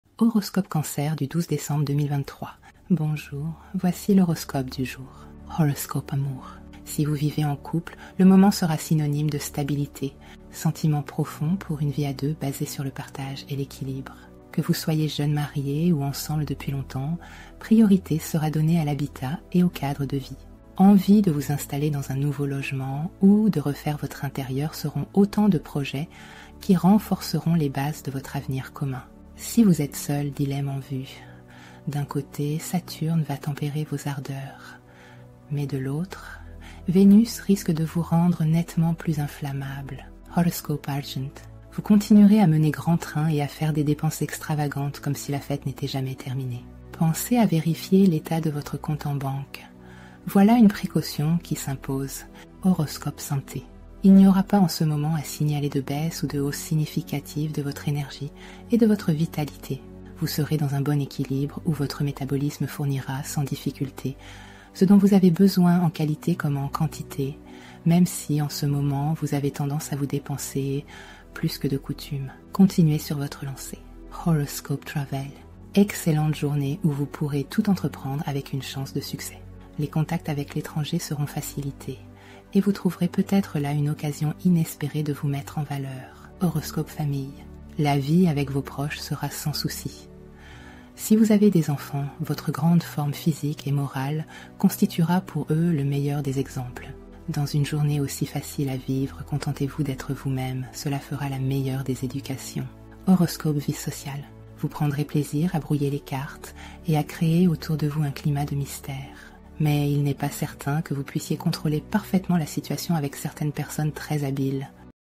Horoscope Cancer du 12 Décembre 2023
0:00 Intro
0:06 horoscope amour
0:41 horoscope argent
0:54 horoscope santé
1:16 horoscope travail
1:29 horoscope famille
1:46 horoscope vie sociale